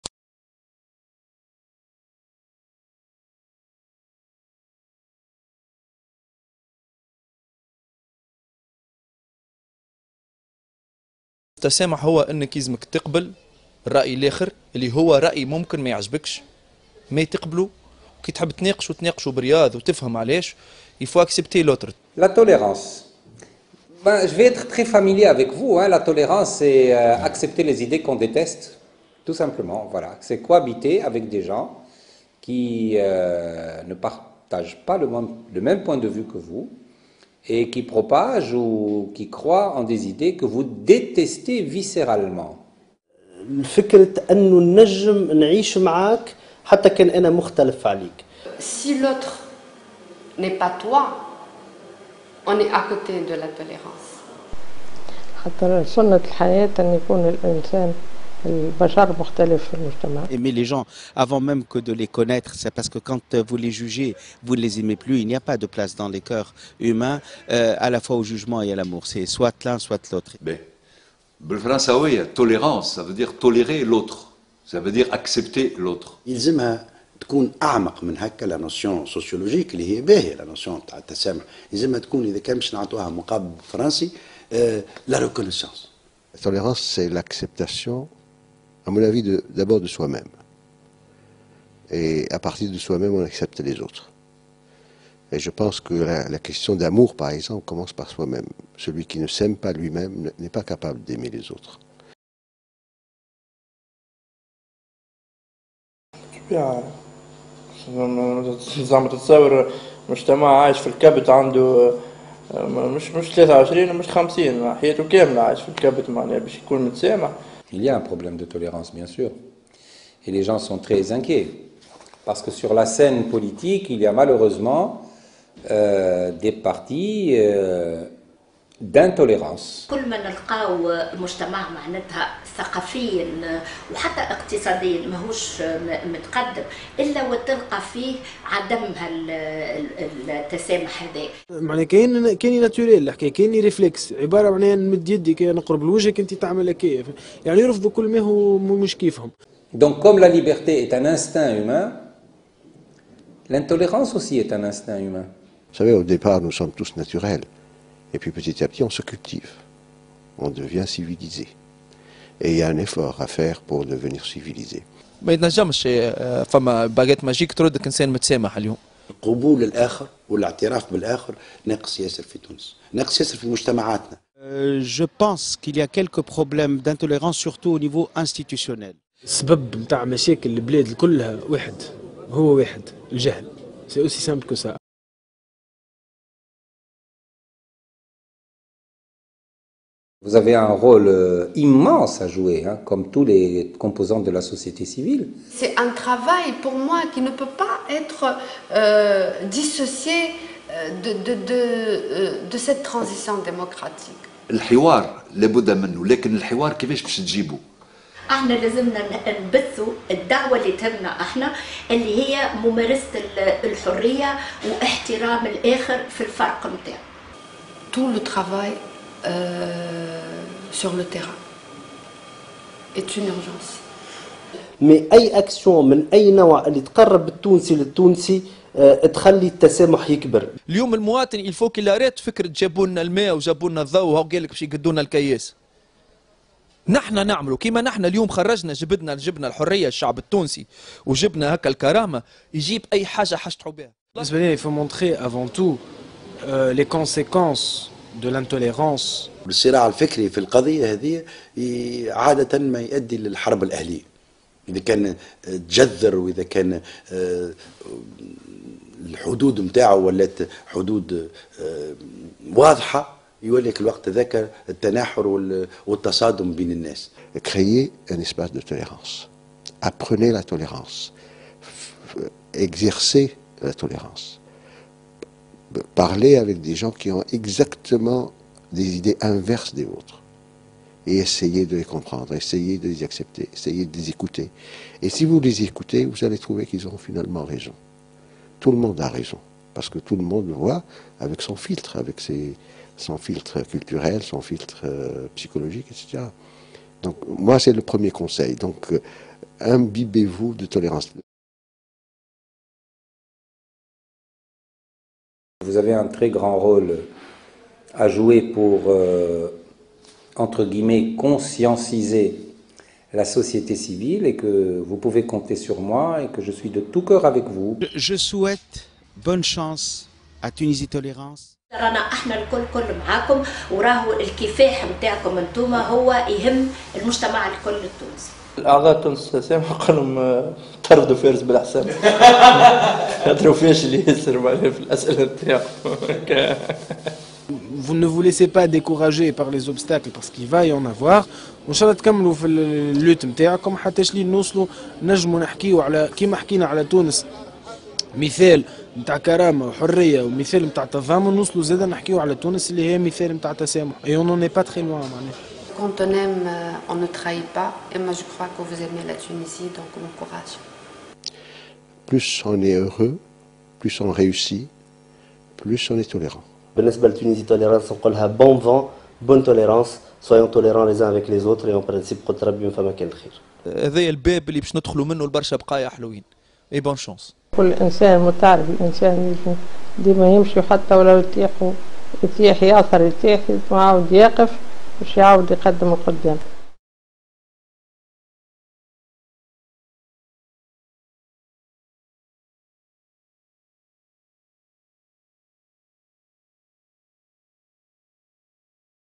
Parlons Tolérance!
De nombreuses personnalités de la société civile nous parlent de la tolérance en Tunisie : Yadh Ben Achour, Youssef Seddik, Dr Ridha Kamoun, Leyla Toubel, Salma Baccar, Hichem Rostom, Bendir Man, Haythem El Mekki (ByLasko), Elyes Gharbi, Anouar Moalla...